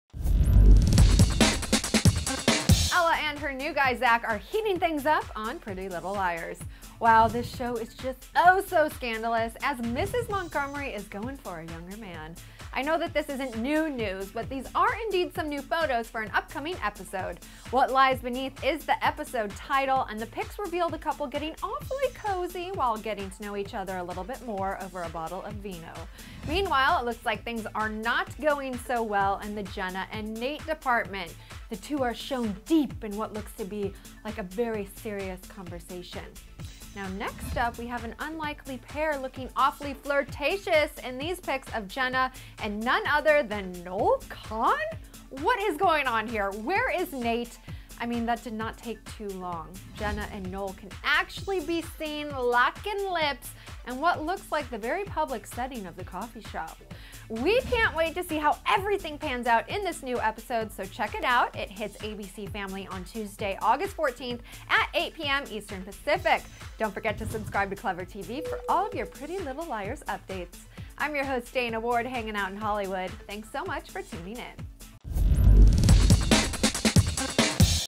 New Romances in "Pretty Little Liars" 3x10 - "What Lies Beneath"
Ella and her new guy Zach are heating things up on Pretty Little Liars. Wow, this show is just oh-so-scandalous, as Mrs. Montgomery is going for a younger man. I know that this isn't NEW news, but these are indeed some new photos for an upcoming episode.  "What Lies Beneath," is the ep title and the pics reveal the couple getting awfully cozy while getting to know each other over a bottle of vino. Meanwhile, it looks like things aren't going so well in the Jenna and Nate department. The two are shown deep in what looks to be a serious conversation. Next up we have an unlikely pair looking awfully flirtatious in these pics of Jenna and none other than--- NOEL KAHN? What is going on here? Where's Nate? That didn't take long ---Jenna and Noel can be seen locking lips in what looks like the very public setting of the coffee shop. We can't wait to see how everything pans out! Be sure to check out the new episode will be airing Tuesday, August 14th at 8 PM eastern/pacific and don't forget to subscribe to ClevverTV for all of your PLL updates!

Hosted by: Dana Ward